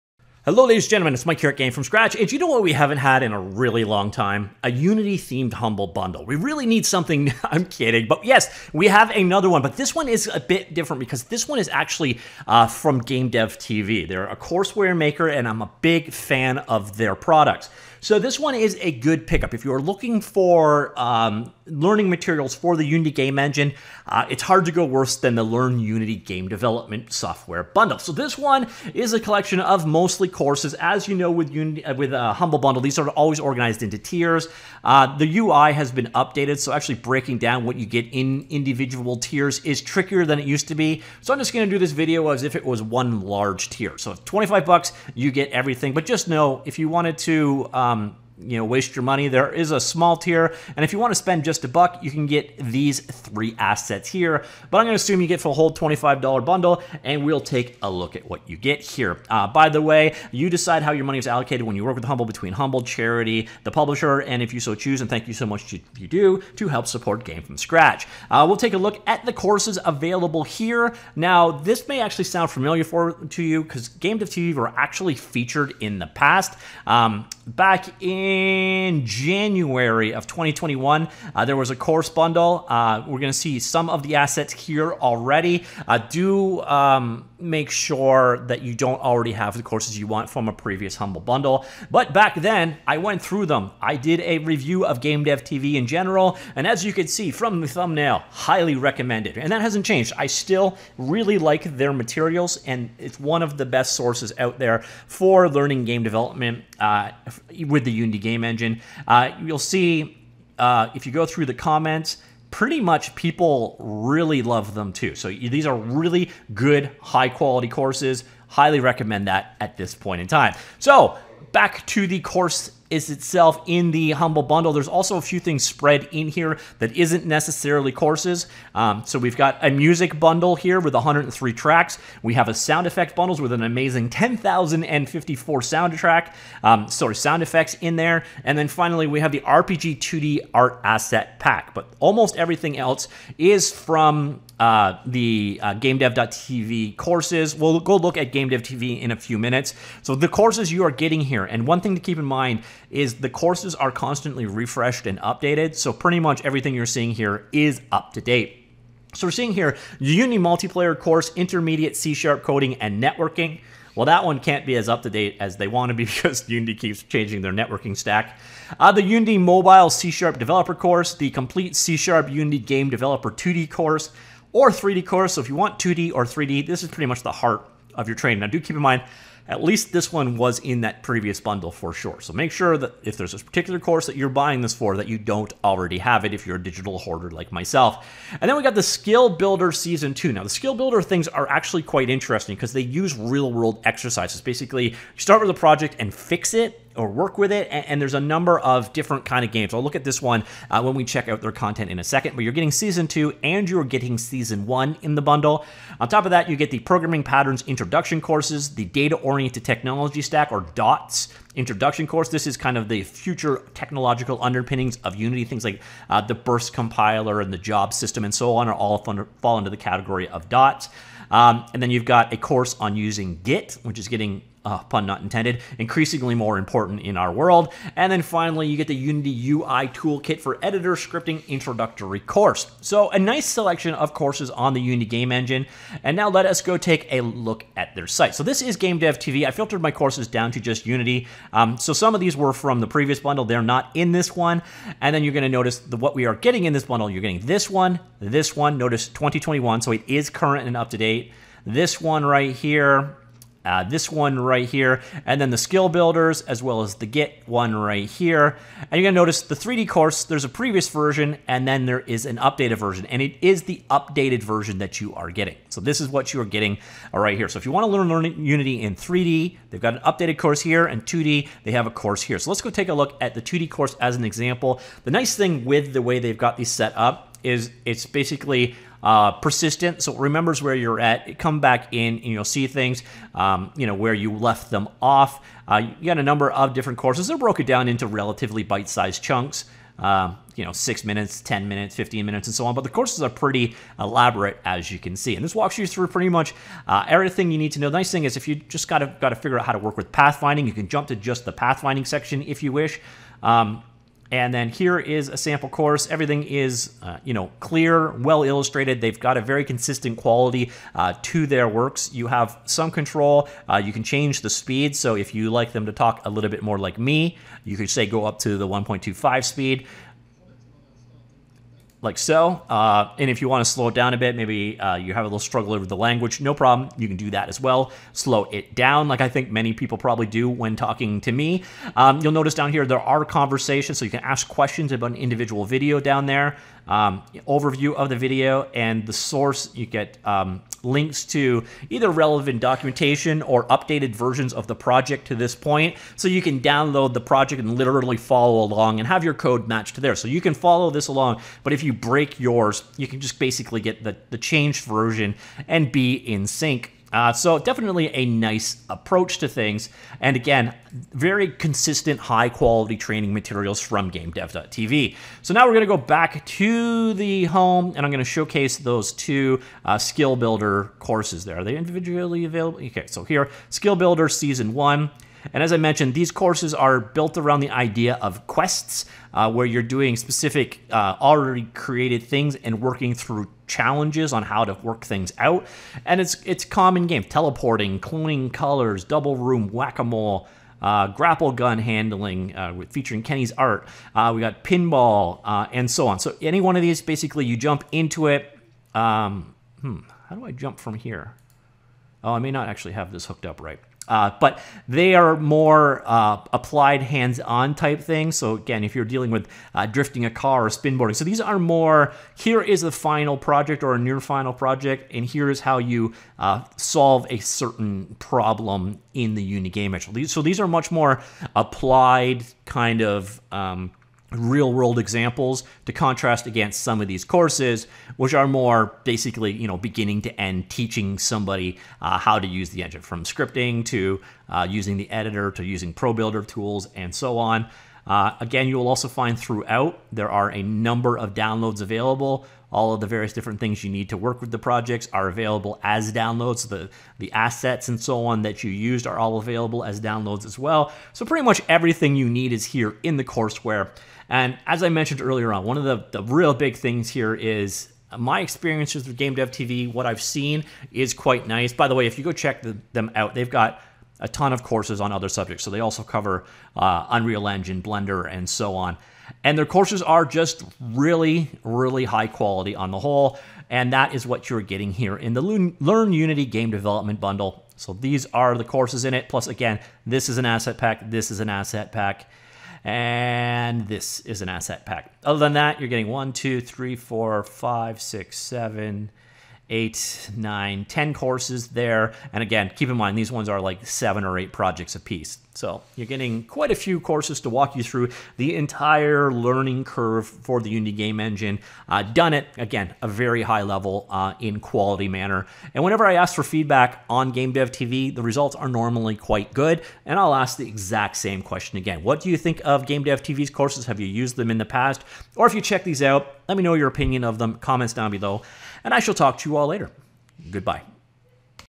Learn Unity with the Excellent GameDev.Tv Humble Bundle
There is another Humble Bundle of interest for game developers, the Learn Unity Game Development Bundle, a collection of up to date Unity courses by GameDev.TV.  Additionally the bundle contains a sprite, music and sound FX asset pack to help get you going.